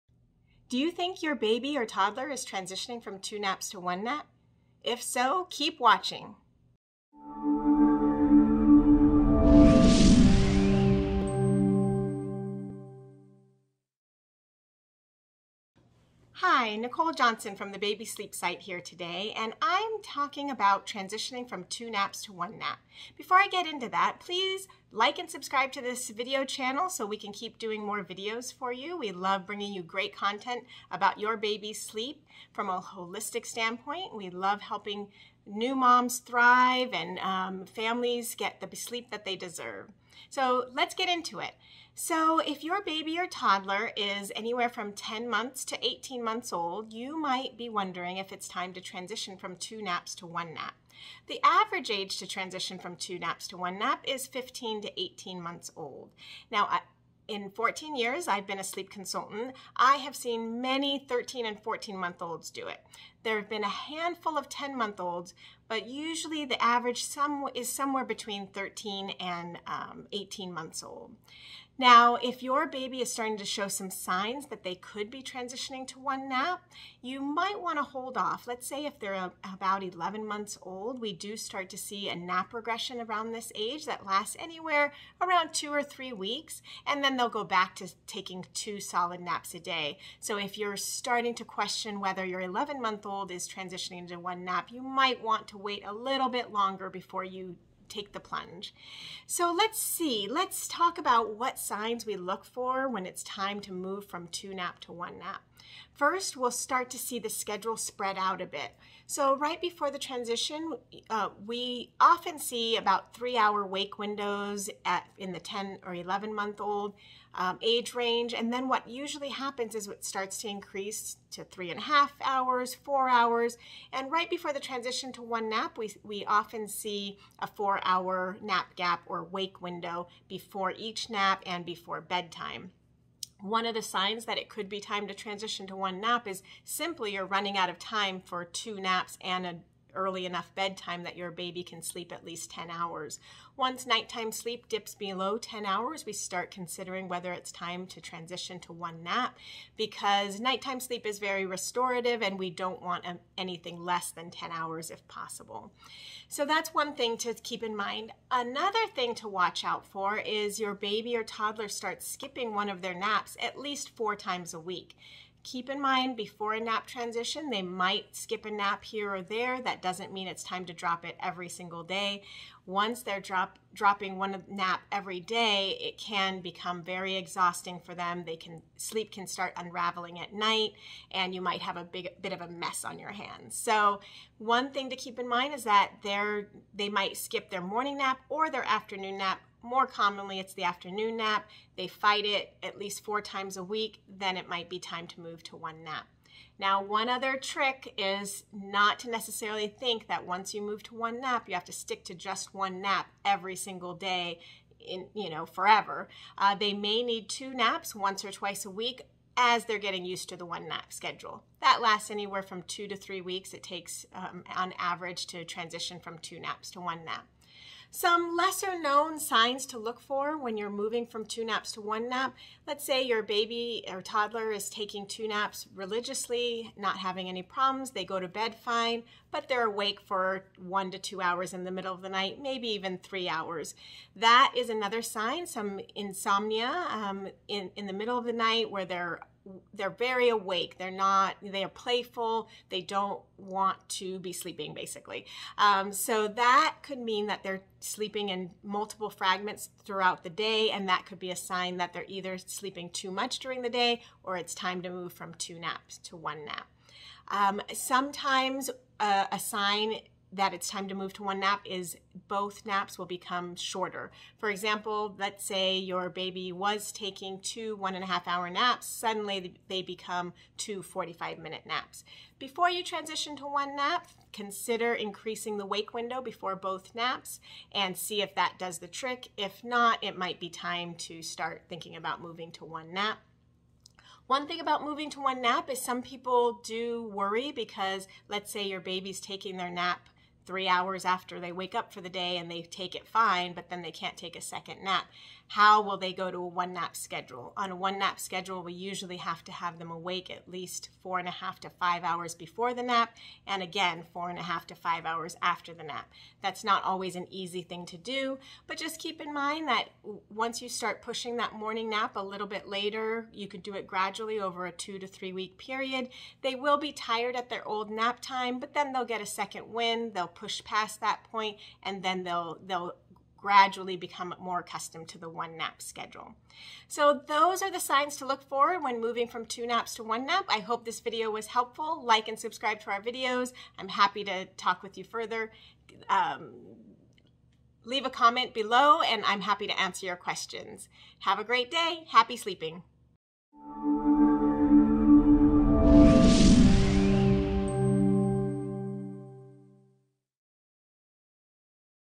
Toddler Transitioning to One Nap? Learn the Signs to Look For
Toddlers transition from two naps to one nap at 10 months to 18 months old. Learn the signs to look for from Nicole Johnson, lead sleep consultant of The Baby Sleep Site.